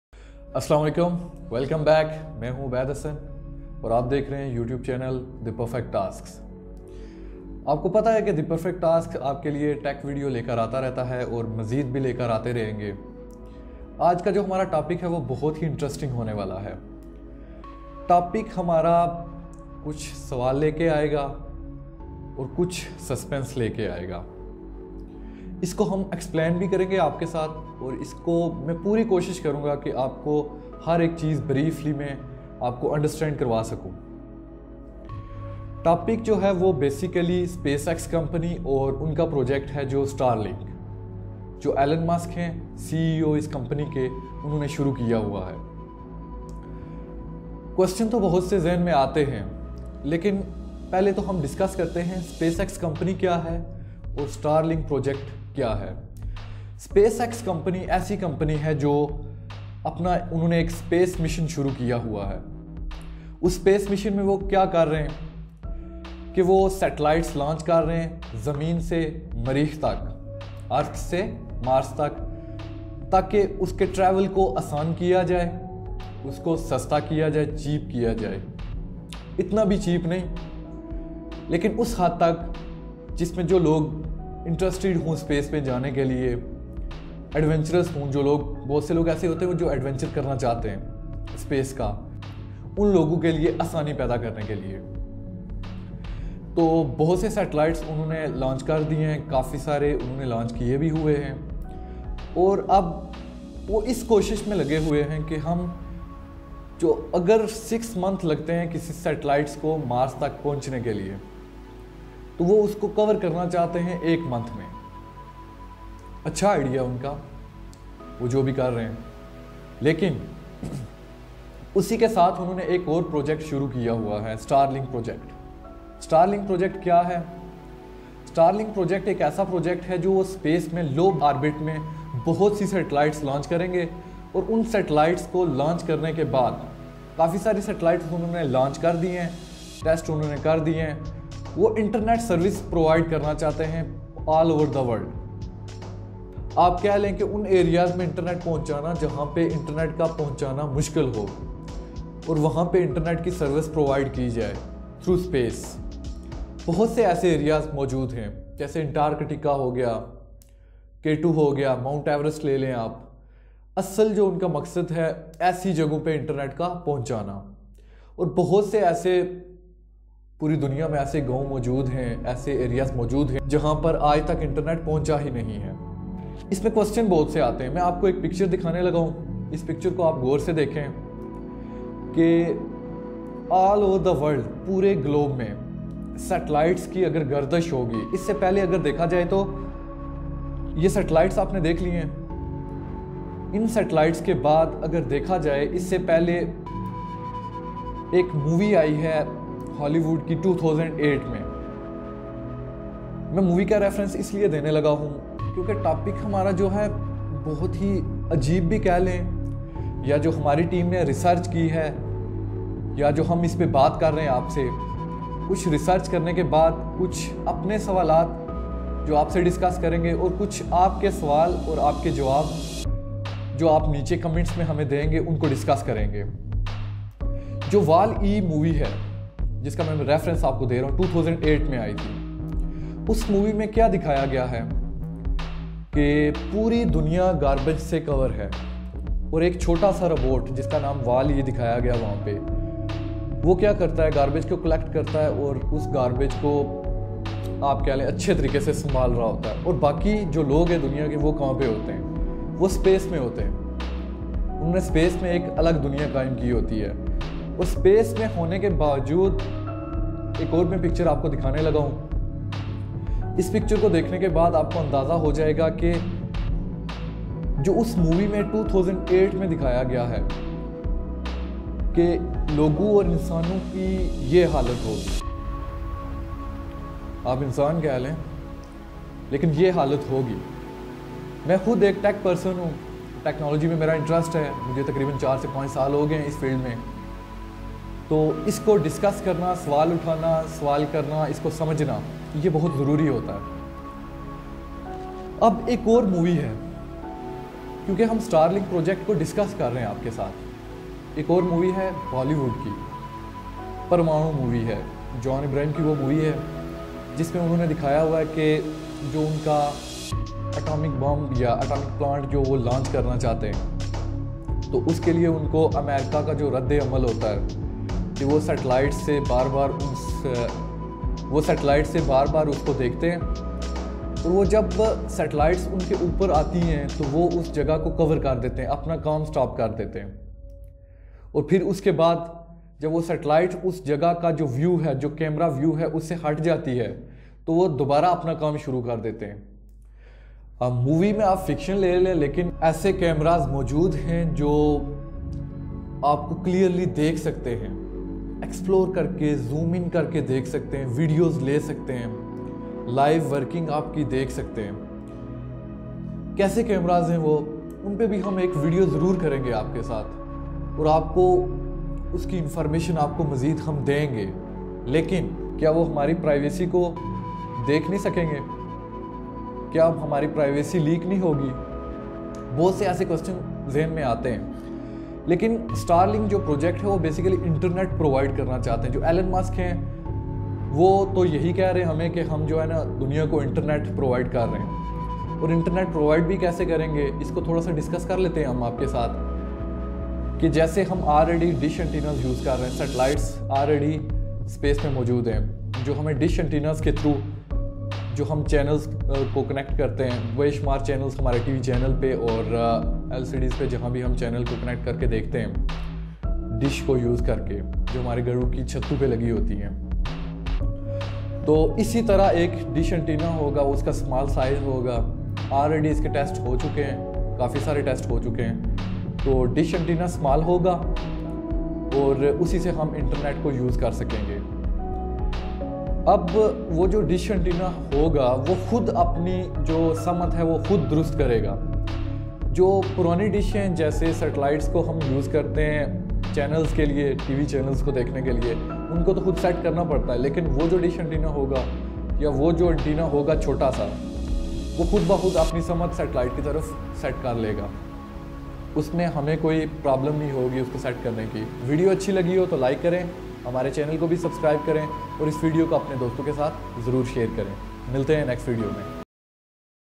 Space X | Starlink Project | ElonMusk Projects Explanation and Myths
CEO of ✅Space X company ElonMusk is doing a project related to the internet. the name of the project is✅Starlink in which ElonMusk trying to provide fast internet through space or you can say that project is already started Soo wait and watch What's happening in coming decades in the world. We are here to provide every piece of information related to technology facts and figures of technology Hope you will remain connected with us😀.